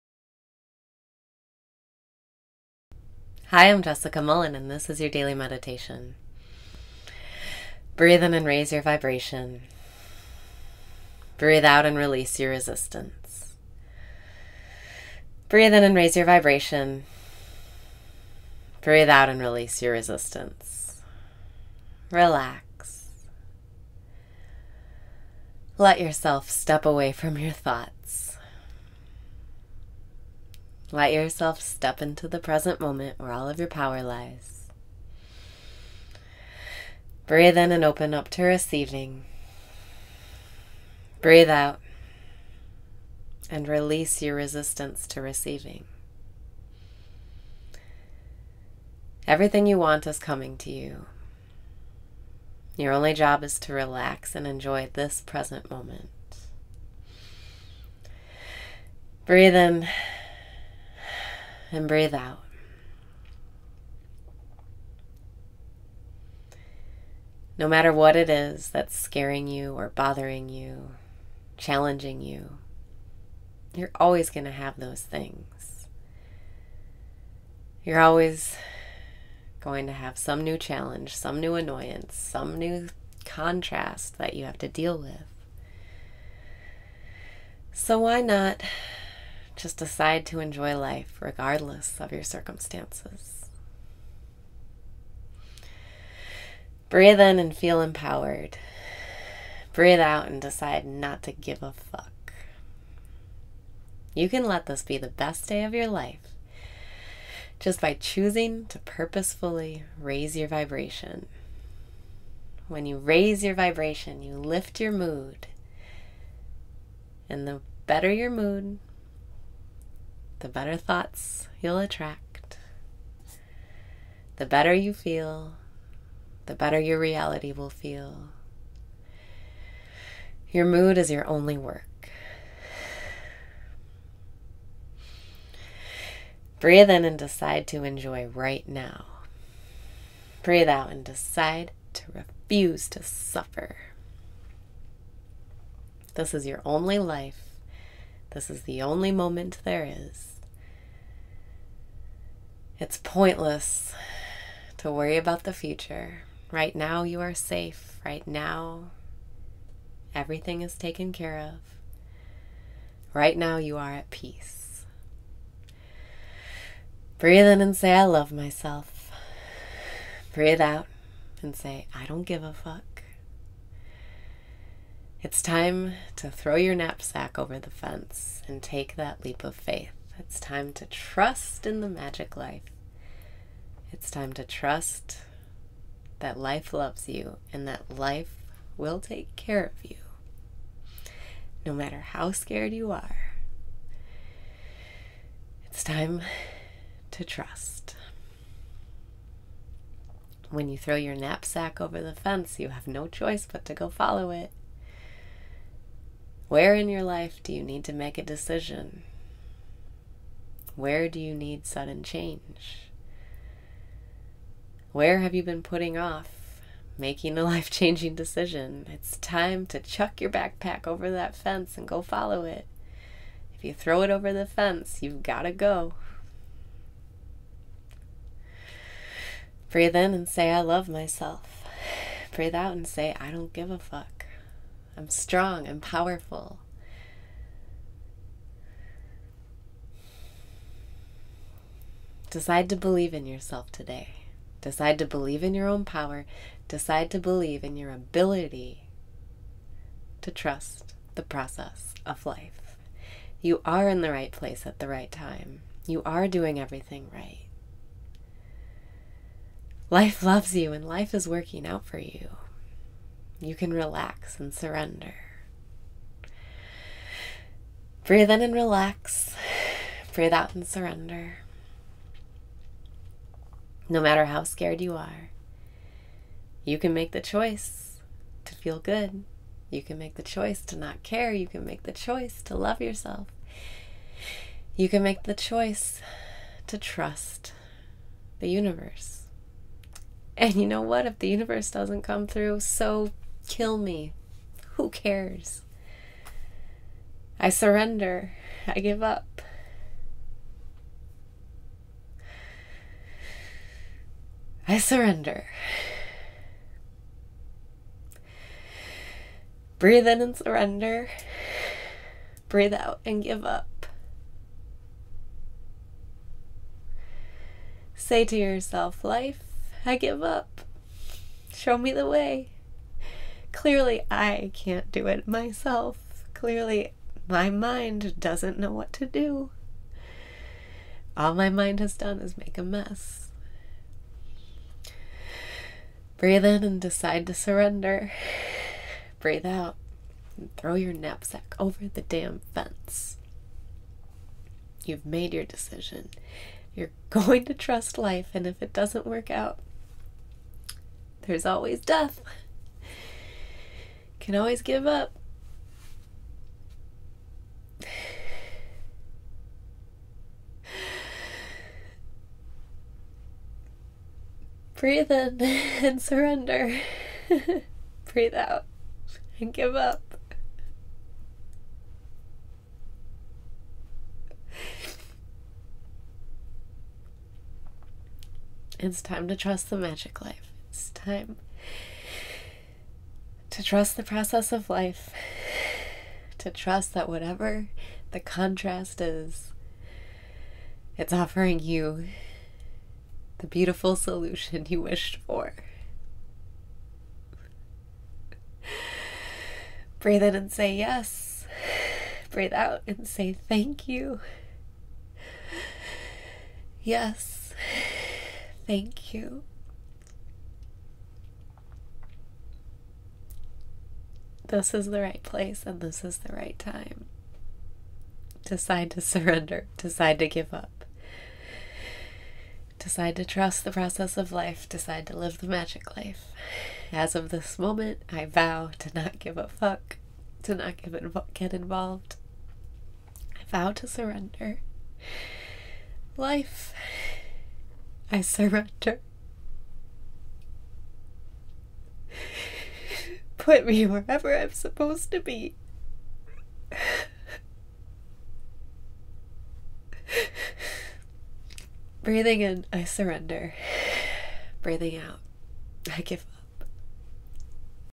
Daily Meditation - Give Up
Relax. Let yourself step away from your thoughts. Let yourself step into the present moment where all of your power lies.

Breathe out and release your resistance to receiving

Everything you want is coming to you. Your only job is to relax and enjoy this present moment.

Breathe in
Breathe out

No matter what it is that’s scaring you or bothering you—challenging you—you’re always going to have those things. You’re always going to have some new challenge, some new annoyance, some new contrast that you’re going to have to deal with. So why not decide to enjoy life regardless of your circumstances?

Breathe in and feel empowered
Breathe out and decide not to give a fuck!

You can let this be the best day of your life, just by choosing to purposefully raise your vibration. When you raise your vibration, you lift your mood. The better your mood, the better thoughts you’ll attract. The better you feel, the better your reality will feel. Your mood is your only work.

Breathe in and decide to enjoy right now.
Breathe out and decide to REFUSE TO SUFFER!

This is your only life. This is the only moment there is. It’s pointless to worry about the future. Right now you are safe. Right now everything is taken care of. Right now you are at peace.

It’s time to throw your knapsack over the fence and take that leap of faith. It’s time to trust in the magick life. It’s time to trust that life loves you and that life will take care of you, no matter how scared you are. It’s time to trust. When you throw your knapsack over the fence you have no choice but to go follow it. Where in your life do you need to make a decision? Where do you need sudden change? Where have you been putting off making a life-changing decision? It’s time to chuck your backpack over that fence and go follow it! If you throw it over the fence you’ve got to go!

Breathe out and say “I don’t give a fuck! I’m strong! I’m powerful!”

Decide to believe in yourself today. Decide to believe in your own power. Decide to believe in your ability to trust the process of life. You are in the right place at the right time. You are doing everything right. Life loves you and life is working out for you! You can relax and surrender.

No matter how scared you are, you can make the choice to feel good. You can make the choice to not care. You can make the choice to love yourself. You can make the choice to trust the universe. And you know what, if the universe doesn’t come through, so kill me! Who cares! I surrender! I give up! I surrender.

Breathe in and surrender
Breathe out and give up

Say to yourself, “Life, I give up! Show me the way! Clearly I can’t do it myself. Clearly my mind doesn’t know what to do! All my mind has done is make a mess”

Breathe in and decide to surrender
Breathe out and throw your knapsack over the damn fence

You’ve made your decision. You’re going to trust life and if it doesn’t work out, there’s always death! You can always give up!

It’s time to trust the magick life. It’s time to trust the process of life, to trust that whatever the contrast is, it’s offering you the beautiful solution you wished for.

Yes!

This is the right place and this is the right time. Decide to surrender. Decide to give up. Decide to trust the process of life. Decide to live the magick life. As of this moment I vow to not give a fuck, to not get involved. I vow to surrender. Life, I surrender. Put me wherever I’m supposed to be.

Breathing in I surrender
Breathing out I give up